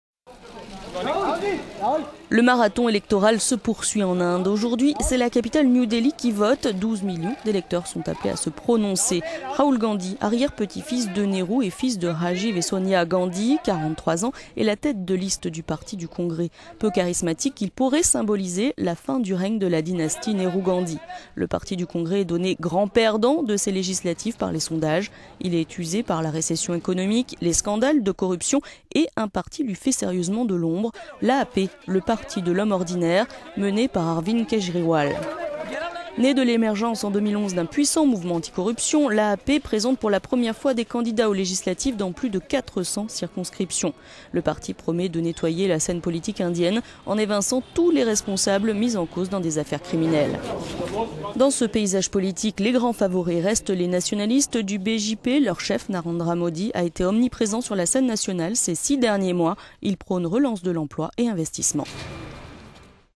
Législatives en Inde, Raul Gandhi vote
Le marathon électoral se poursuit en Inde. Aujourd'hui, c'est la capitale New Delhi qui vote. 12 millions d'électeurs sont appelés à se prononcer pour des législatives historiques.

Raul Gandhi, arrière-petit-fils de Nehru et fils de Rajiv et Sonia Gandhi, 43 ans, est la tête de liste du Parti du Congrès.
Peu charismatique, il pourrait symboliser la fin du règne de la dynastie Nehru-Gandhi.

Le Parti du Congrès est donné grand perdant de ces législatives par les sondages. Au pouvoir depuis 20...